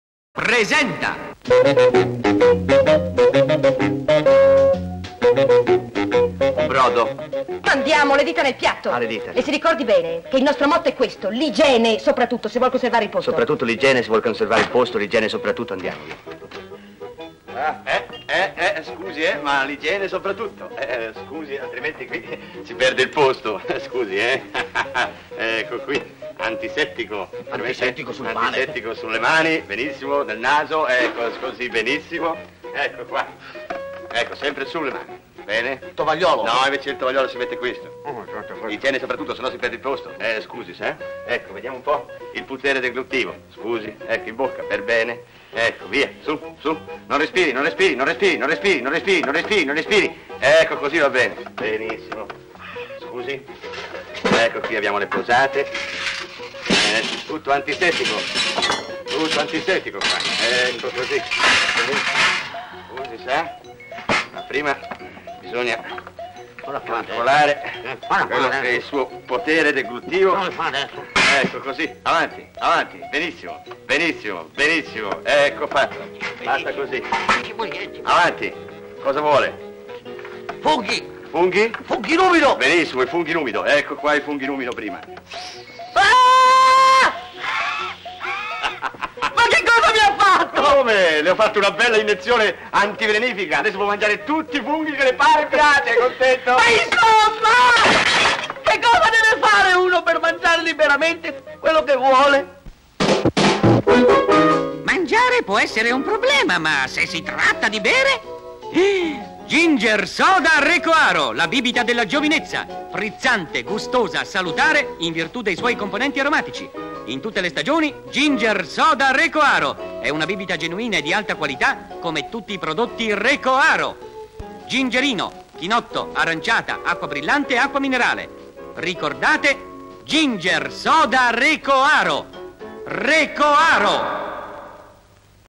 Acqua brillante Recoaro Il cameriere ex precettore il cameriere igienista
Spot Acqua brillante Recoaro Il cameriere ex precettore il cameriere igienista, casa di produzione Derby film, anno prima messa in onda 1960